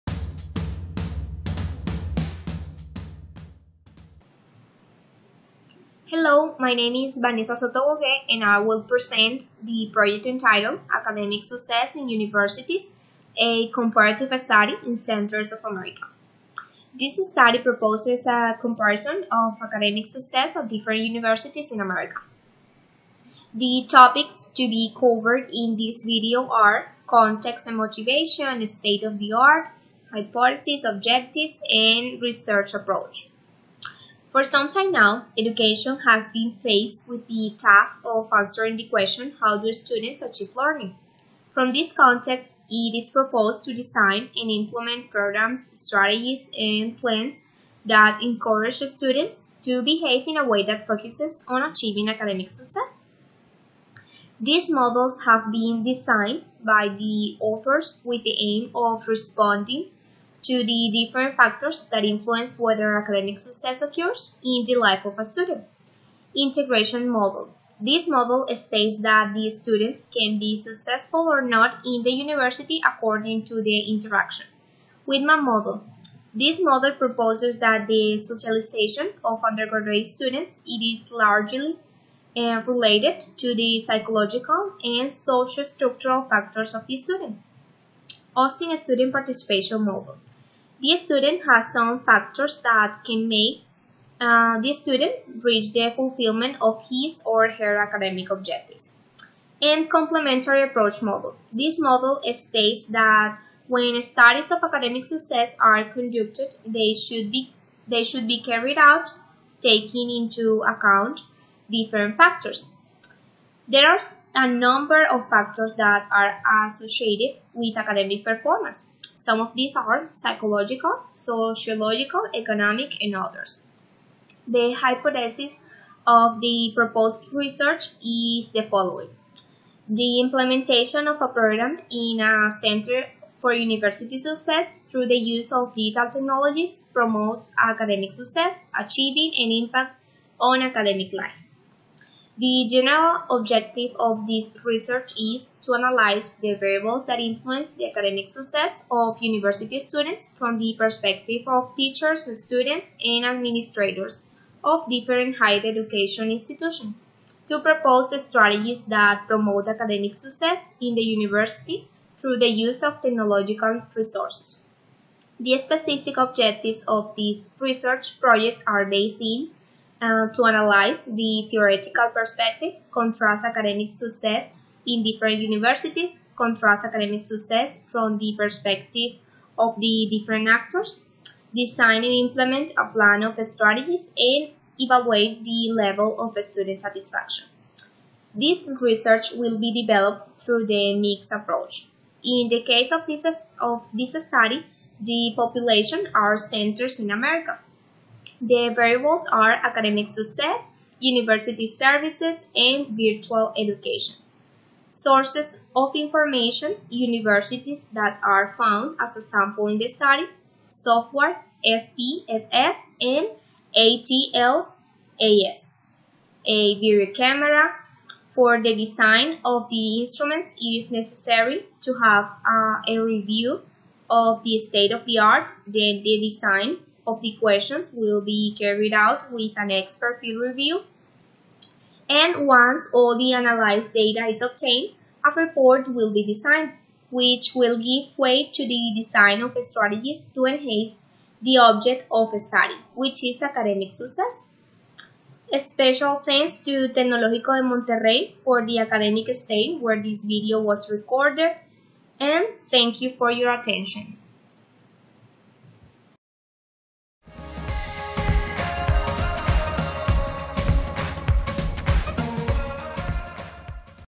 Academic success in universities: a comparative study in Centers of America
Authors: Vanessa Soto Gogué, Maria Soledad Ramírez-Montoya, Ángel Hernando Gómez and Paula Renés-Arellano

How to cite this work:
Soto Gogué, V., Ramírez-Montoya, M. S., Hernando Gómez, A. and Renés-Arellano, P. (2020). Academic success in universities: a comparative study in Centers of America.. In F. J. García-Peñalvo (Eds.), Proceedings of the 8th International Conference on Technological Ecosystems for Enhancing Multiculturality (TEEM 2020) (Salamanca, Spain, October 21-23, 2020)